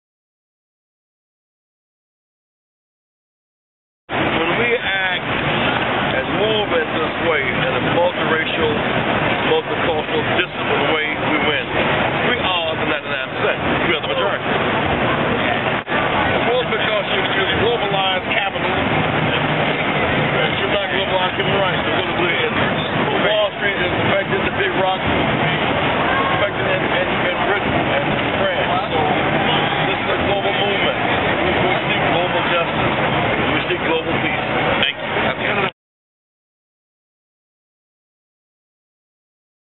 Jesse Jackson at Occupy Wall Street
Jesse Jackson, African-American civil rights activist, speaks at Occupy Wall Street on 18 October 2011.